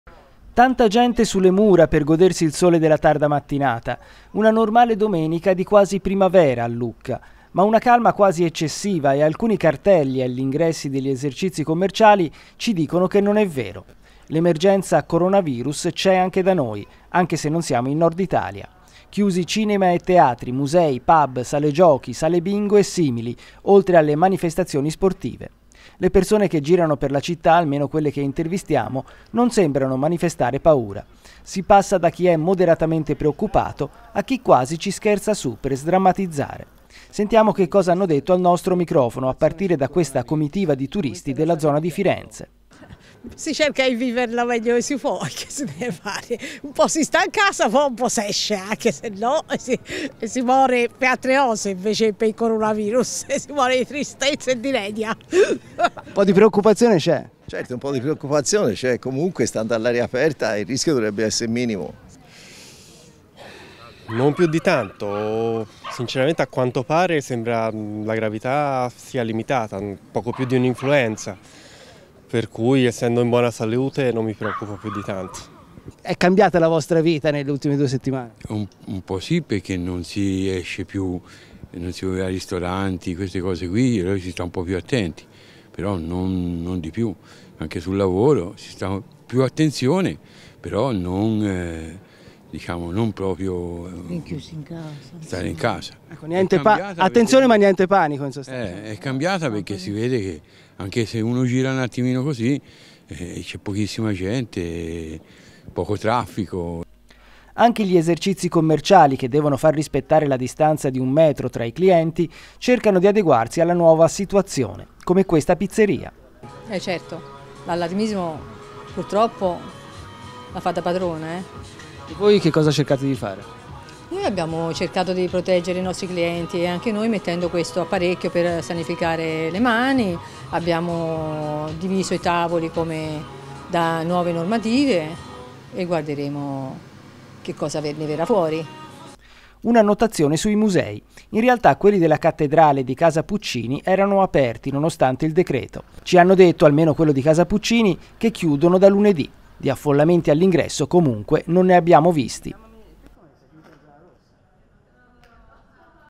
A passeggio sulle mura; la "normale" domenica a Lucca, pensando al coronavirus
LUCCA - Sole, e tanta gente sulle Mura per godersi il sole della tarda mattinata. Una normale domenica di quasi primavera a Lucca. Ma una calma quasi eccessiva e alcuni cartelli agli ingressi degli esercizi commerciali ci dicono che non è vero